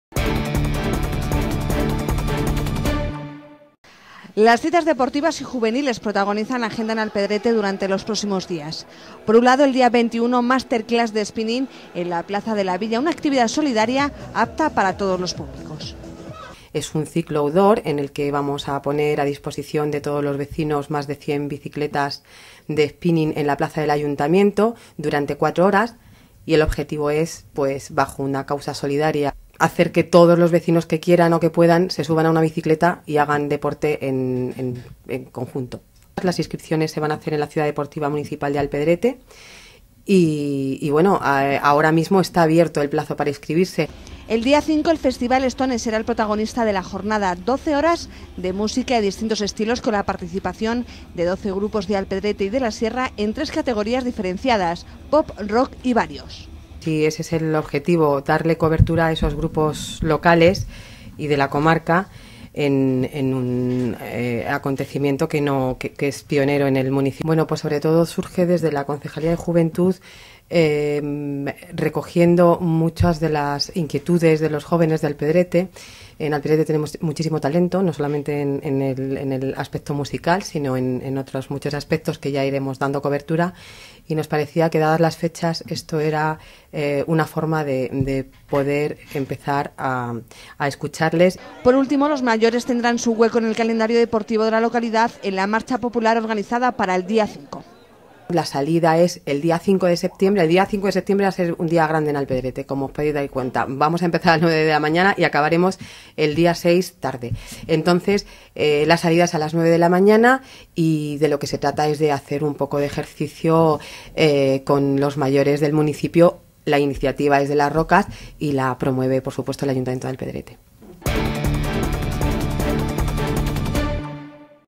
Actividades deportivas y juveniles en Alpedrete
17/08/2015. Actividades deportivas y juveniles en Alpedrete, nos lo ha contado la Concejala de Deporte y Juventud del Ayuntamiento de Alpedrete Ana Isabel Balandin.